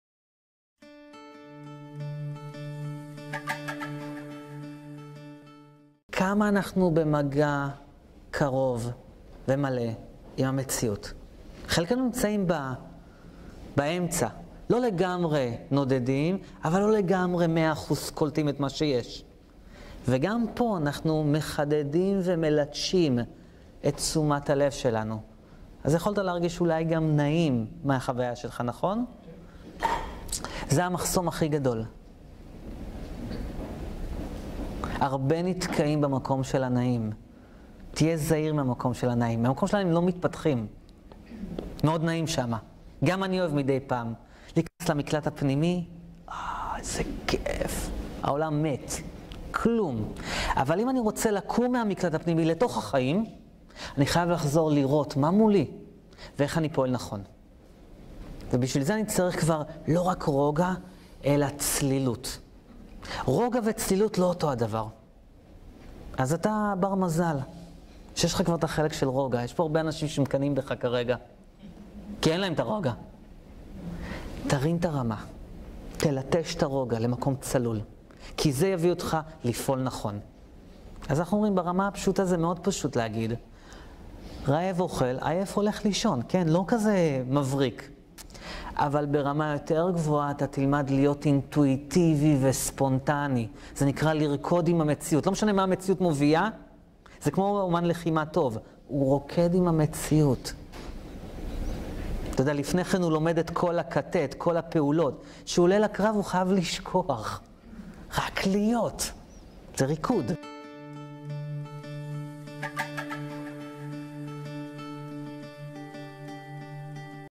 איך אתה שומר את המיינד שלך
לכולנו יש אוצר והאוצר הזה נקרא תשומת לב.
היכן אתה שומר את תשומת הלב?
תרגול תשומת לב מפתח שלווה וצלילות.
בואו ללמוד, ללטש ולהשתמש באוצר הגדול שיש בנו.
לפרטים נוספים נא להתקשר עם איילת דניאל בטלפון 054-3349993